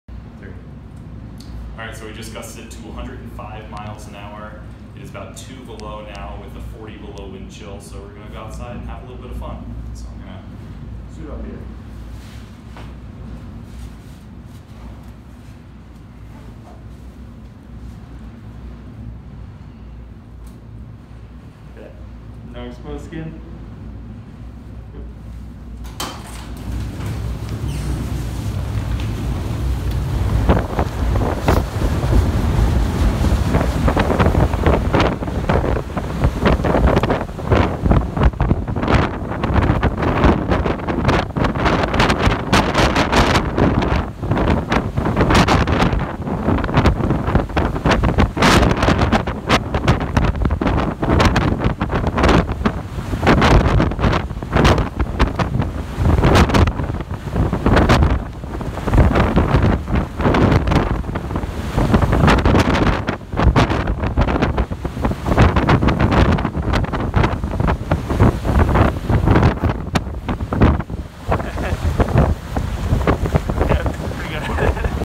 2017 11 10 105 mph Winds
Weather Observer Tom Padham and Intern Greg step outside for a bit of fun on the coldest day so far this season, with winds gusting up to 105 mph and a wind chill approaching -40°F!